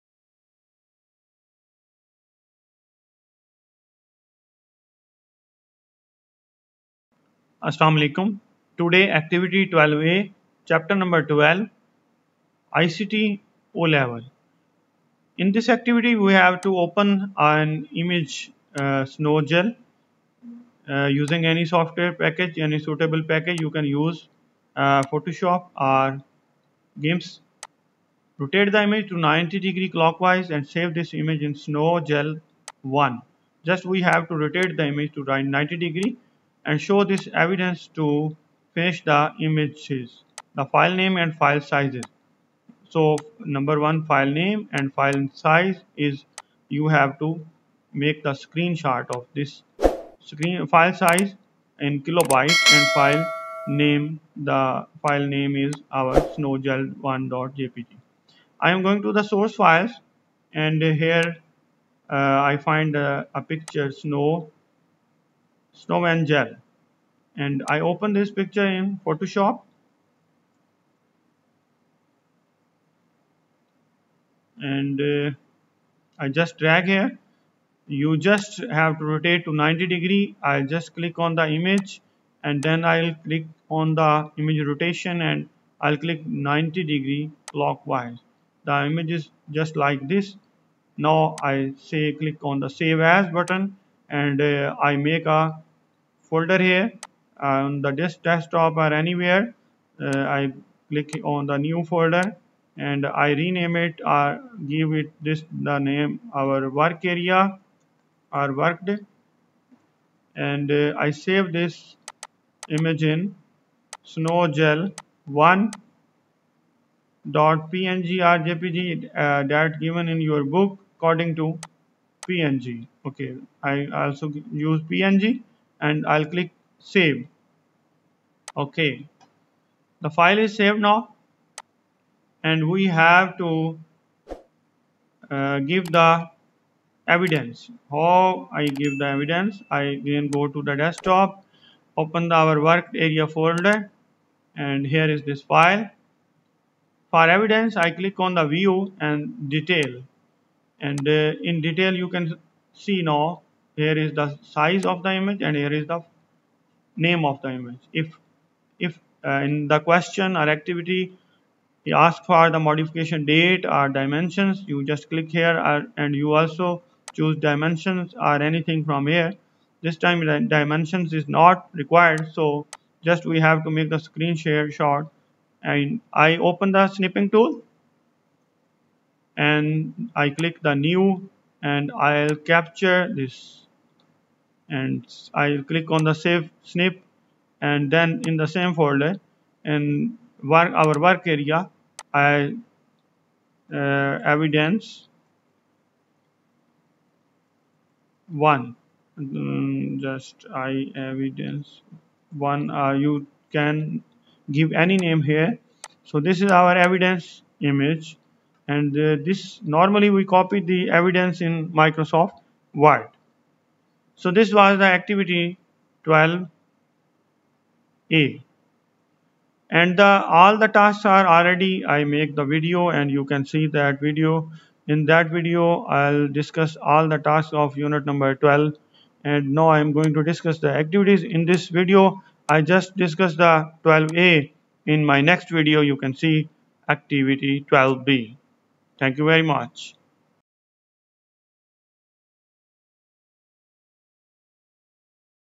Cambridge IGCSE ICT Third Edition 0417 Activity 12a Chapter 12
Cambridge IGCSE Information and Communication Technology Third Edition: Watson, David, Brown, Graham
Hodder Education
Information and Communication Technology syllabuses (0417/0983).

Cambridge IGCSE ICT Practical chapter 12 Activity 12a

By: Muhammad Imran Hussain Khan